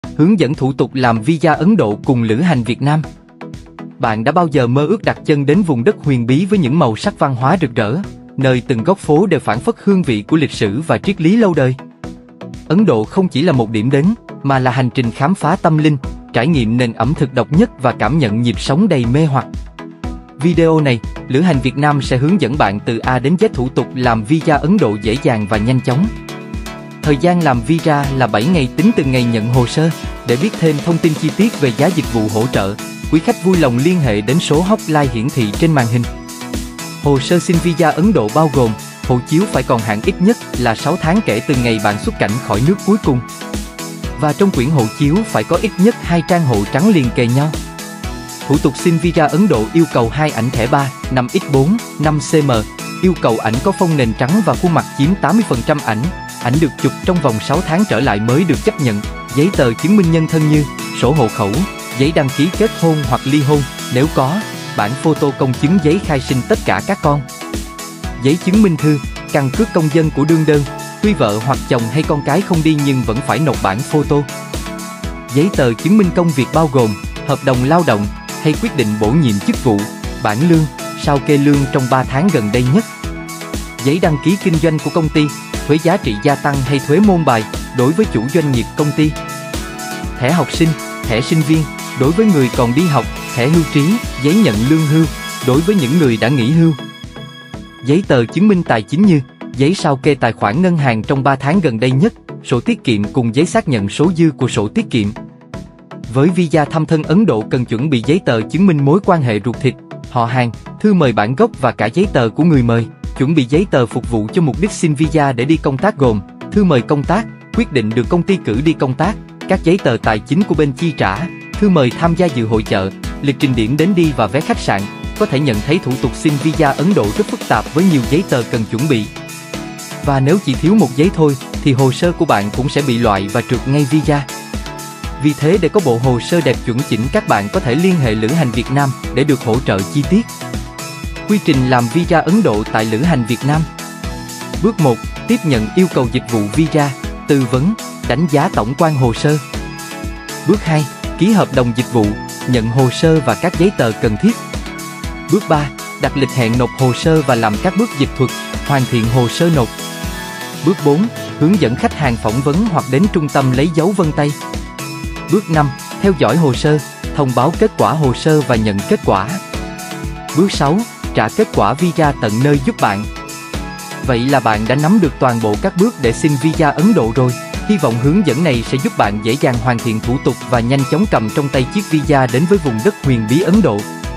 Visa Ấn Độ | Hướng dẫn làm Visa Du lịch Ấn Độ
Hướng dẫn thủ tục làm Visa Du Lịch Ấn Độ đơn giản, nhanh chóng cùng Lữ hành Việt Nam